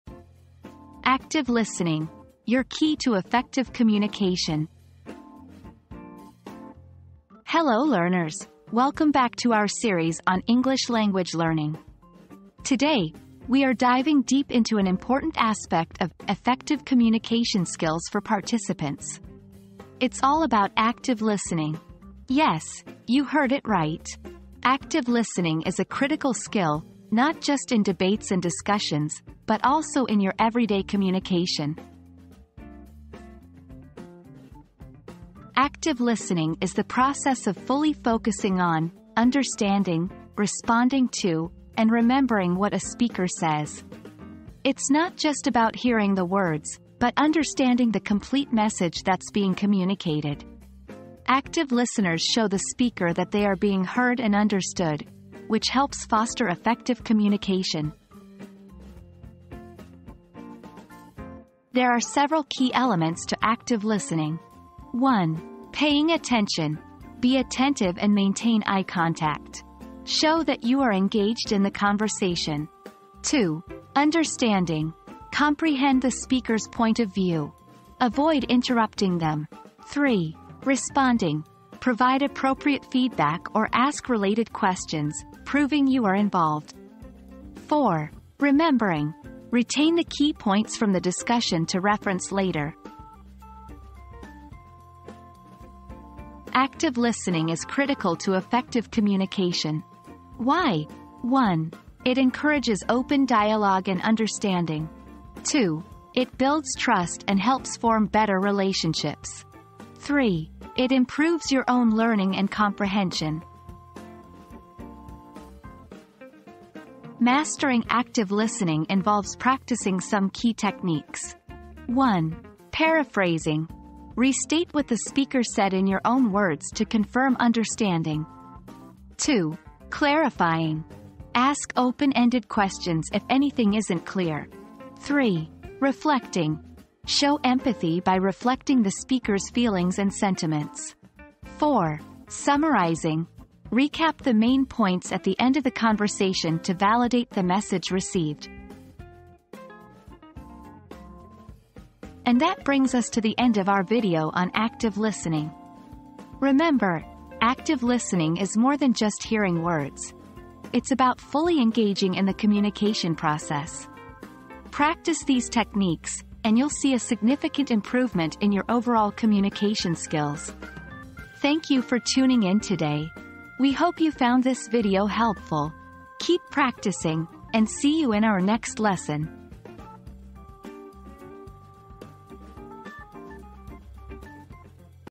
Active Listening: Your Key to Effective Communication
Mastering Active Listening: The Key to Effective Communication • Discover the power of active listening and how it can transform your communication skills. Learn the key elements of active listening, understand its importance, and uncover techniques to become a master listener. Enhance your relationships, build trust, and improve your overall comprehension with active listening.

00:00 • Introduction - Active Listening: Your Key to Effective Communication
00:35 • What is Active Listening?
01:01 • Elements of Active Listening
01:35 • The Importance of Active Listening
01:56 • Techniques for Active Listening